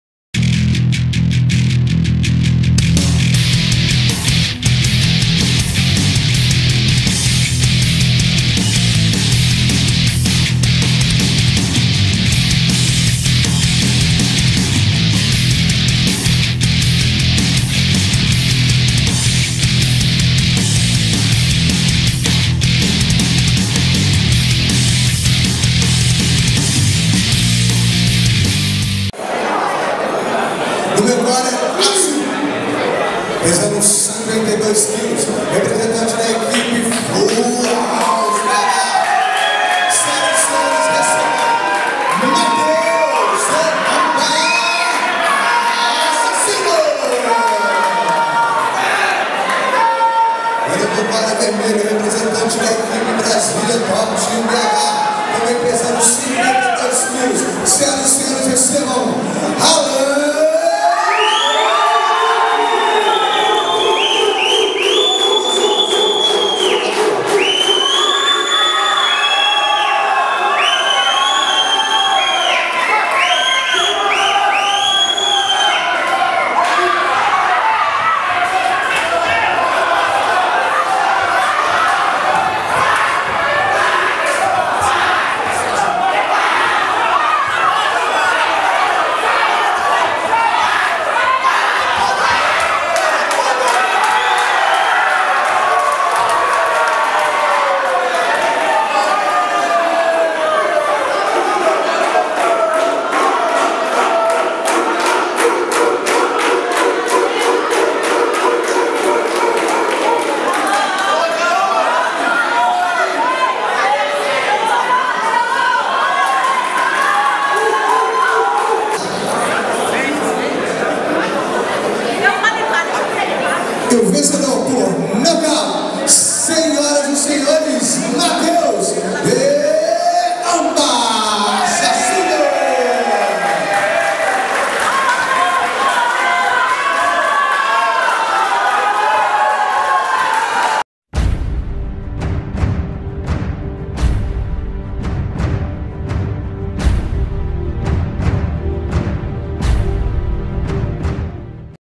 Matheus vs Hallan BH Sparta 5
No combate até 52kg de MMA infantil no BH Sparta 5, Matheus “The Armbar Assassino” (Full House) venceu Hallan Andrade (BTT/BH) no primeiro round por nocaute técnico.